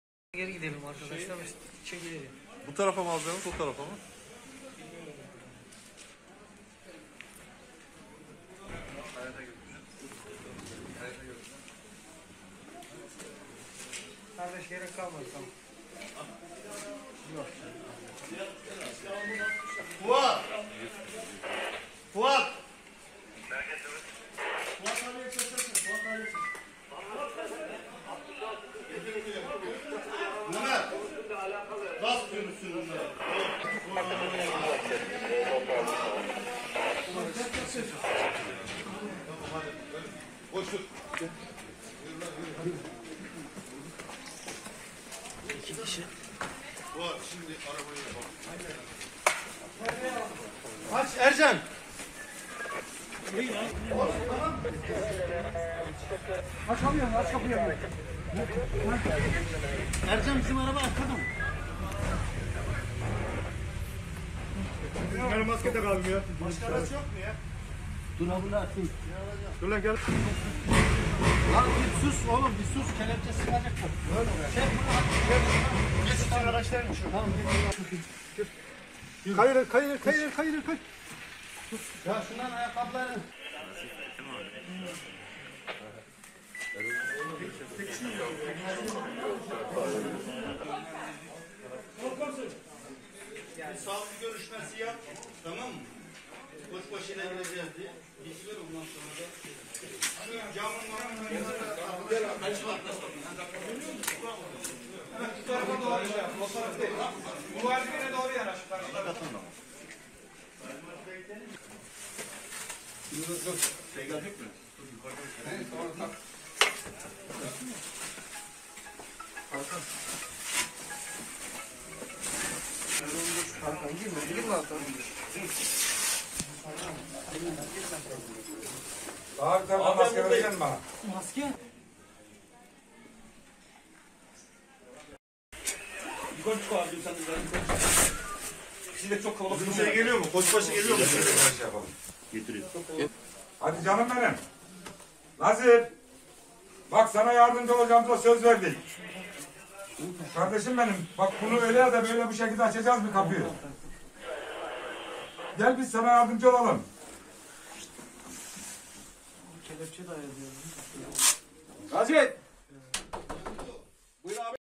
Alacaklıları geldi, pompalı tüfekle kendini eve kilitledi
Bursa'da alacaklıları kapıya dayanan adam tüfekle kendini eve kilitledi. Koç başıyla kırılan kapının içerisinden 2 kişi çıkarıldı.